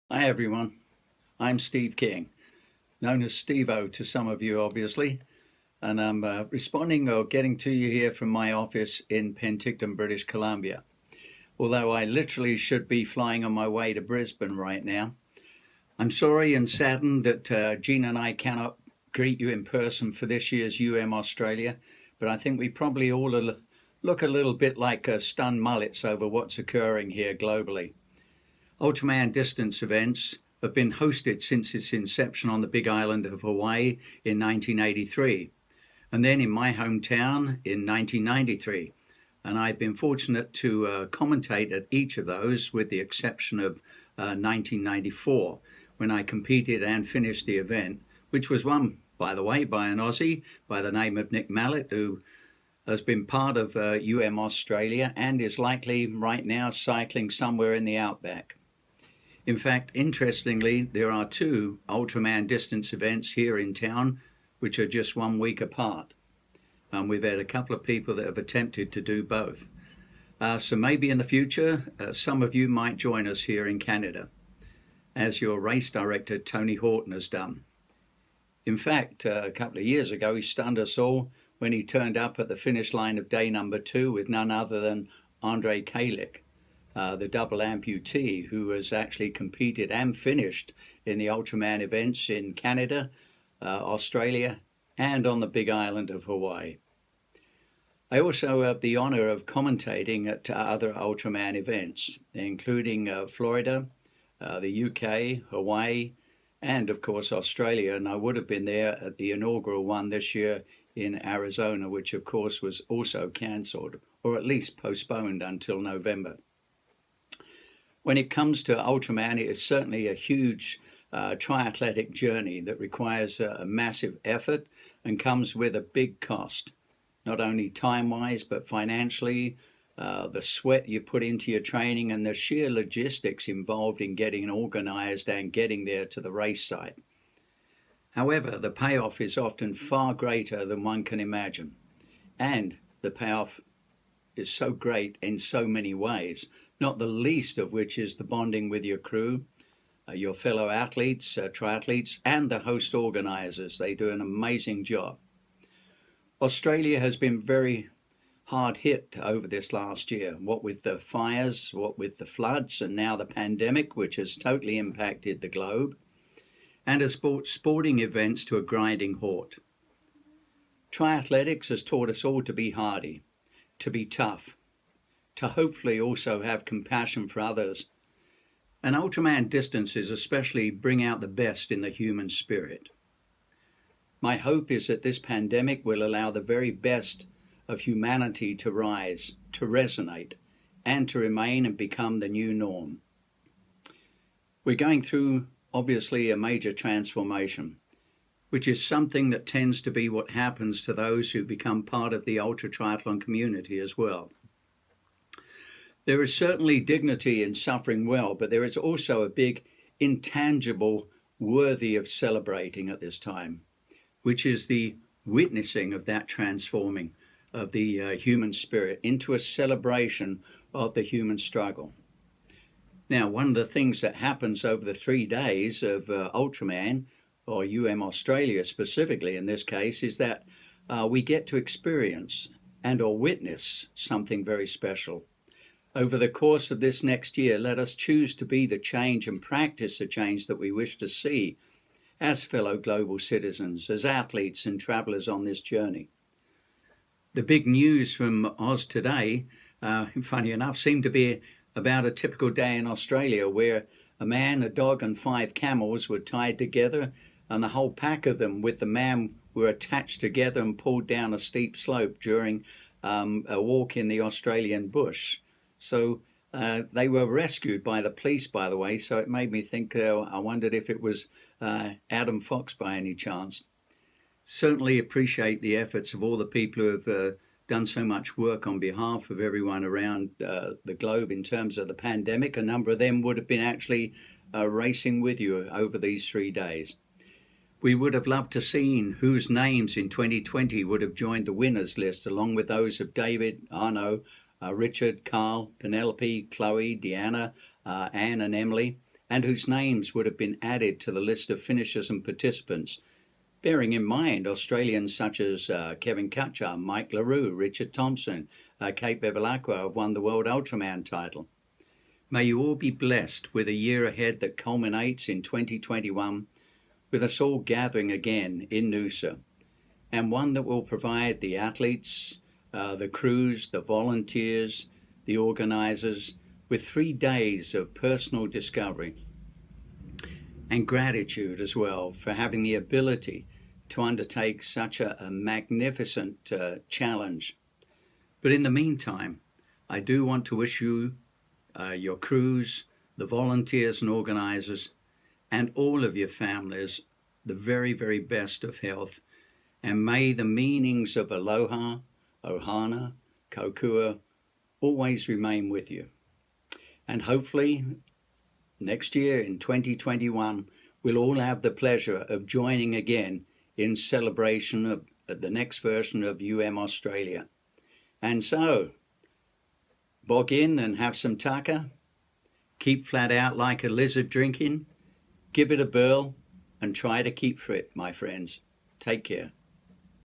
UMAUST 2020 - Steve King
A greeting to those who would have been part of UMAUST 2020 in Noosa